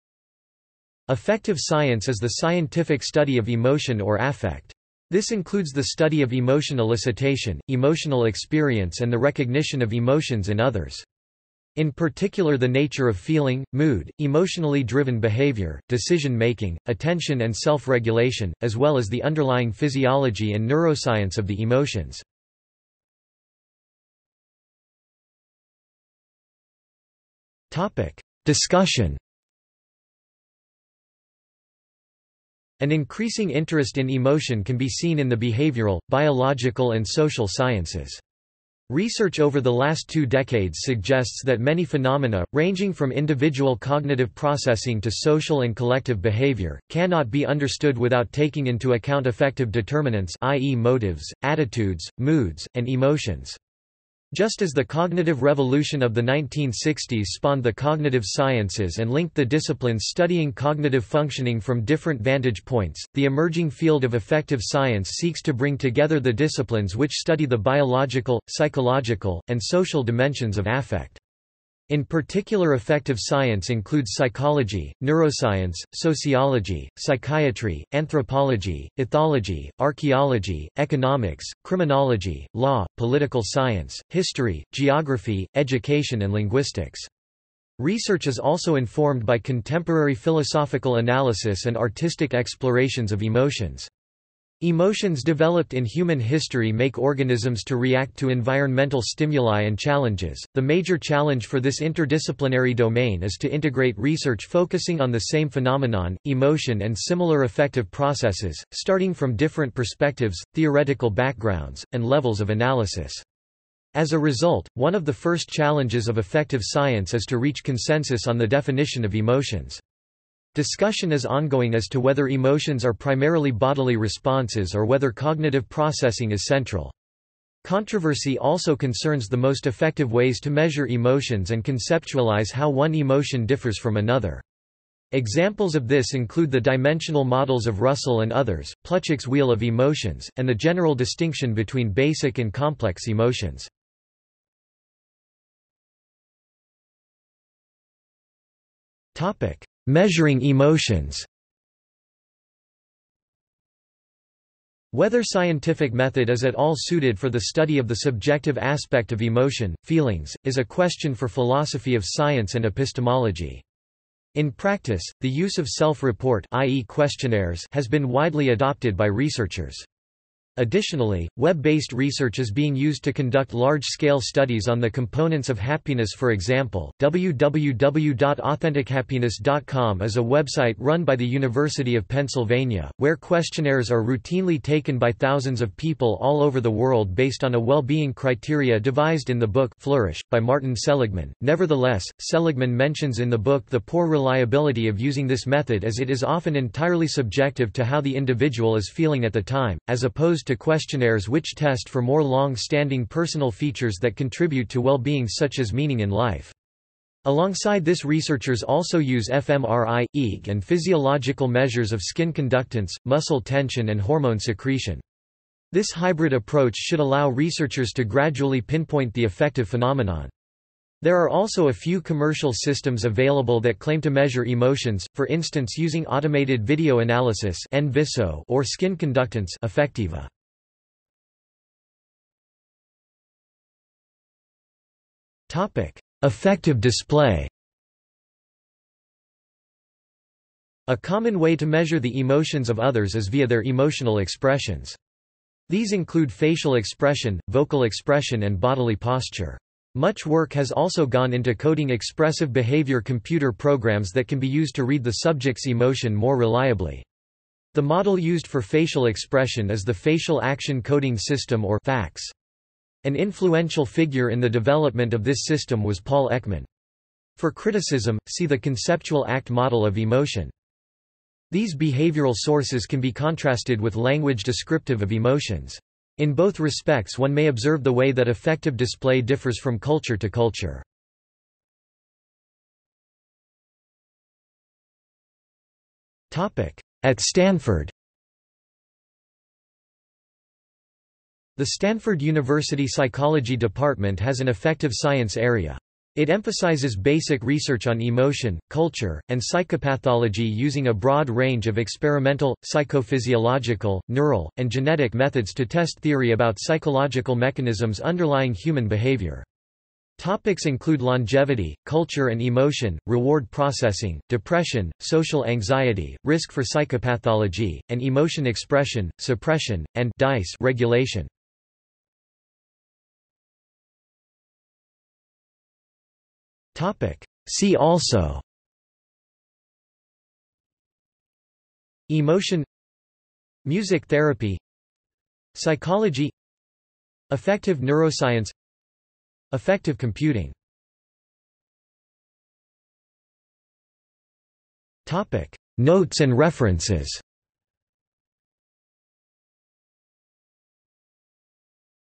Affective science | Wikipedia audio article
00:00:24 1 Discussion
00:02:25 2 Measuring emotions
00:03:52 2.1 Affective display
00:04:42 3 At Stanford
00:05:23 4 See also
00:05:41 5 Notes and references

- Socrates

SUMMARY
Affective science is the scientific study of emotion or affect. This includes the study of emotion elicitation, emotional experience and the recognition of emotions in others. In particular the nature of feeling, mood, emotionally driven behaviour, decision making, attention and self-regulation, as well as the underlying physiology and neuroscience of the emotions.